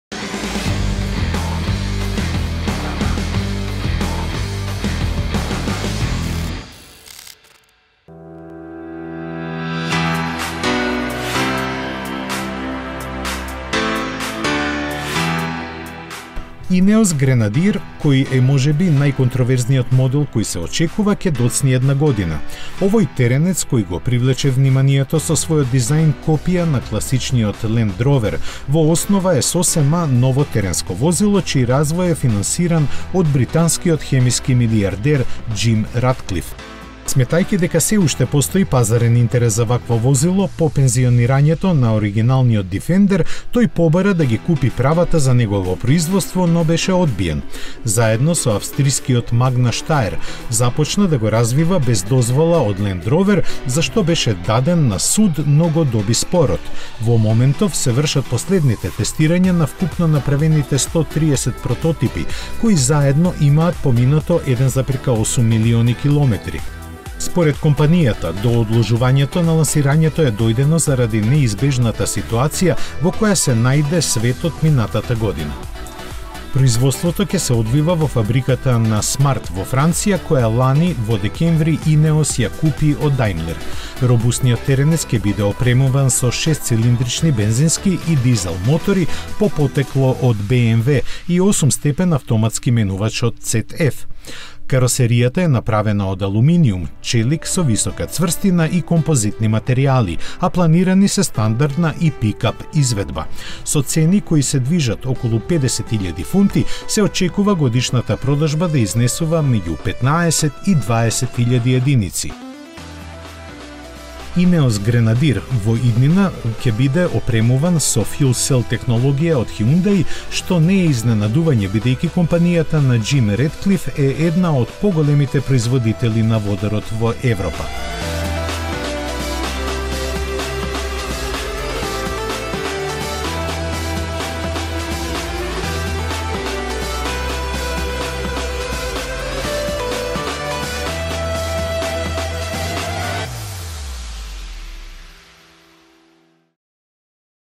Avto Magazin Ineos Grenadier
Реинкарнација на Defender со најмодерна технологија доаѓа вгодина